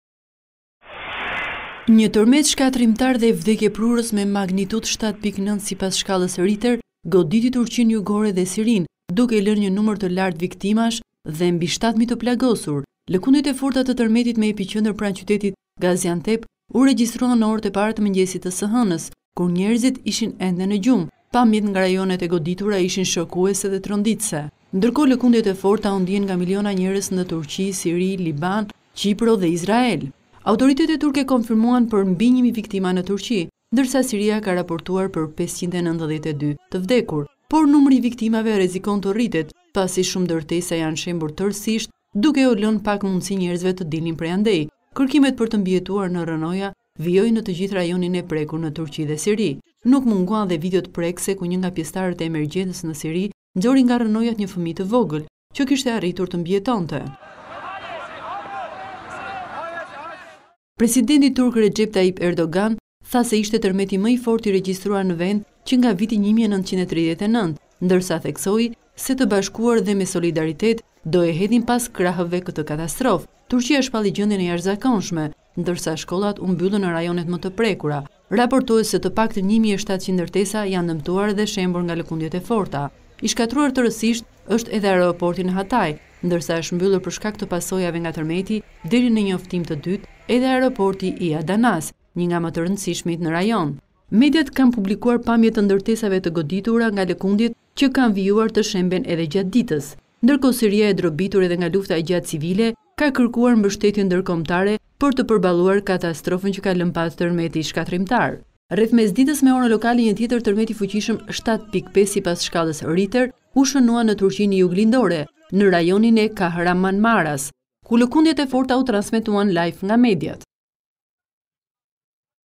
Tërmete të fuqishme trondisin Turqinë,i pari 7.9 ballë në kufirin me Sirinë një tjetër 7.5 në qendër
Një tërmet i fuqishëm 7.9 ballë ka goditur kufirin mes Turqisë jugore dhe Sirisë duke shkaktuar më shumë se 1600 viktima dhe mbi 7000 të plagosur. Lëkundja tronditi banorët pas orës 2 të mëngjesit të së hënës me epiqendër pranë qytetit të Gaziantep me thellësi rreth 7 km. Pasgoditjet e fuqishme s'ndalen, mijëra familje në rrugë! Dy lëkundje të tjera shumë të fuqishme, të magnitudave 7.5 dhe  5.9 të shkallës Rihter, janë regjistruar në Turqinë qendrore.